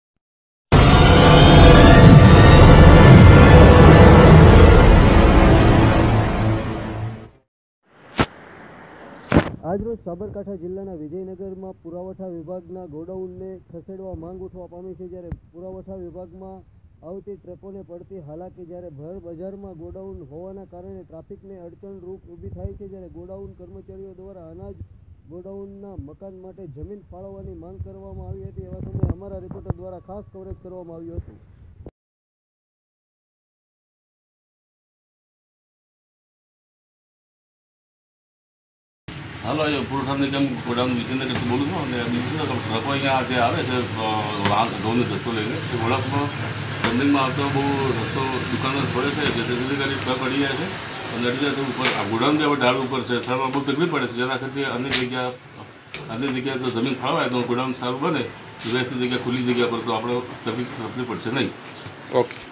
બ્રેકિંગ. વિજયનગર વિજયનગર પુરવઠા વિભાગ ના ગોડાઉન ને ખસેડવા માંગ પુરવઠા વિભાગમાં આવતી ટ્રકો ને પડતી
બ્રેકિંગ. વિજયનગર
વિજયનગર પુરવઠા વિભાગ ના ગોડાઉન ને ખસેડવા માંગ

પુરવઠા વિભાગમાં આવતી ટ્રકો ને પડતી હાલાકી.

ભરબજારમાં ગોડાઉન હોવાના કારણે ટ્રાફિક ને અડચણો ઉભી થાય છે.

ગોડાઉન ના કર્મચારીઓ દ્વારા અનાજના ગોડાઉનના મકાન માટે જમીનની ફાળવવાની માંગ કરી.